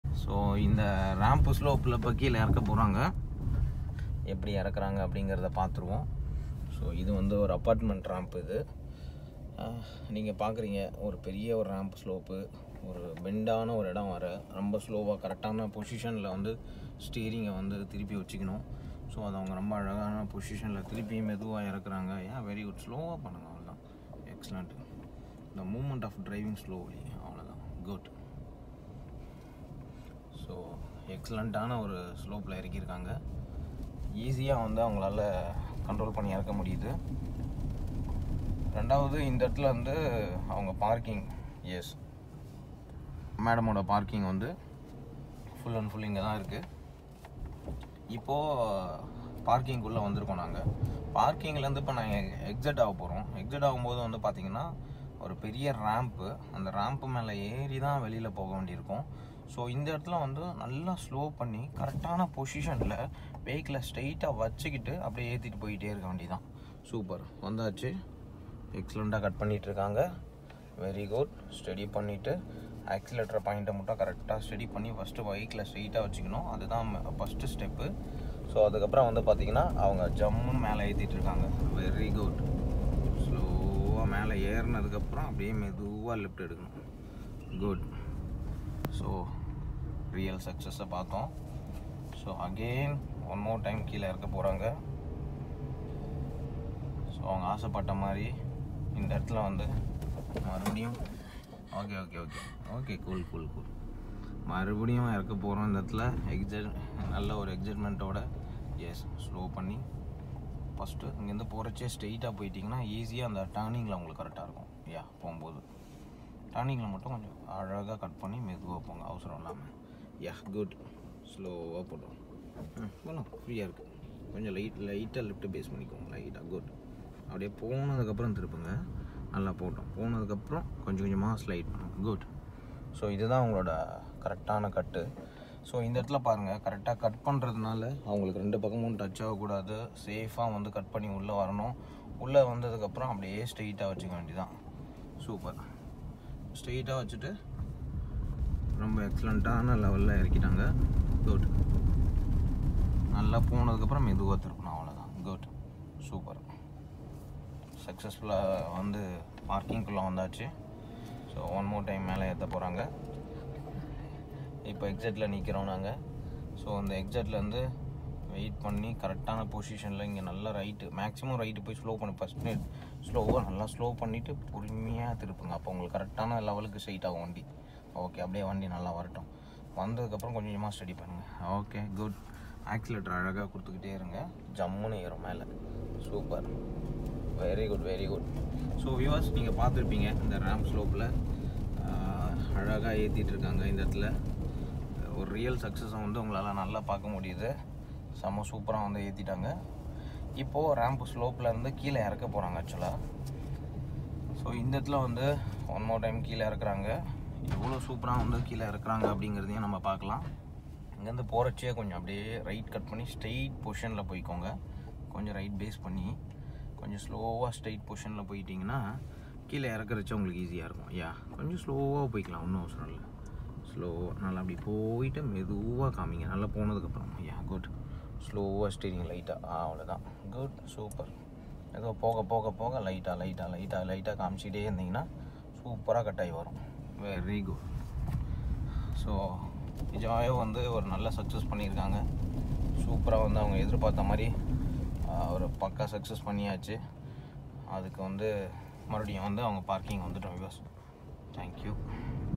How to drive car in slope tamil | Hill Starts With And Without The Handbrake Chennai car trainers
How to reverse out of a parking space parking tips

How to drive a car on slope with clutch control techniques in Tamil Chennai car trainers

To check out the special perks we offer our membersl, click the JOIN button or this link: